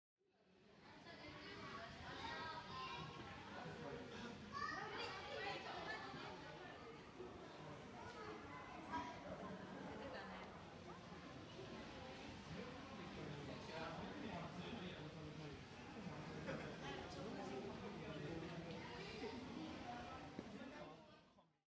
続＊ セマルハコガメ
2016年8月14日　上野動物園 西園 両生爬虫類館（ビバリウム）にて撮影